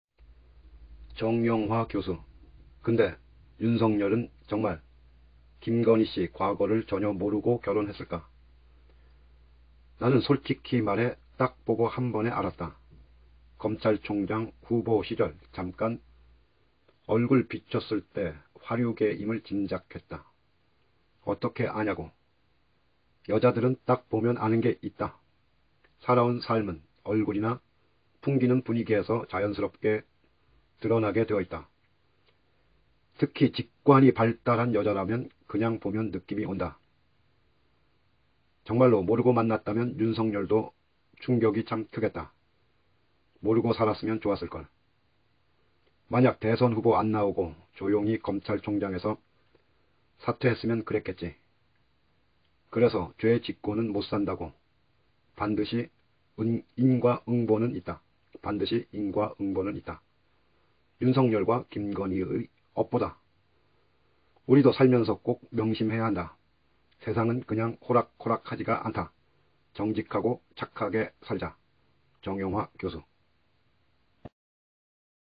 정영화 교수...근데 윤석열은 정말 김건희씨 과거를 전혀 모르고 결혼했을까?나는 솔직히 말해 딱 보고 한 번에 알았다. 검찰총장 후보 시절 잠깐 얼굴 비췄을때 화류계임을 짐작했다.
근데 윤석열은 정말 김건희씨 과거를 전혀 모르고 결혼했을까?

나는 솔직히 말해 딱 보고 한 번에 알았다. 검찰총장 후보 시절 잠깐 얼굴 비췄을때 화류계임을 짐작했다.
어떻게 아냐고?
여자들은 딱 보면 아는게 있다. 살아온 삶은 얼굴이나 풍기는 분위기에서 자연스럽게 드러나게 되어 있다. (특히 직관이 발달한 여자라면 그냥 보면 느낌이 온다.)

정말로 모르고 만났다면 윤석열도 충격이 참 크겠다.
모르고 살았으면 좋았을걸...
만약 대선 후보 안나오고 조용히 검찰총장에서 사퇴했으면 그랬겠지.

그래서 죄짓고는 못산다고, 반드시 인과응보는 있다.
윤석열과 김건희의 업보다.
우리도 살면서 꼭 명심해야 한다.
세상은 그냥 호락호락하지가 않다.
정직하고 착하게 살자!!

정영화 교수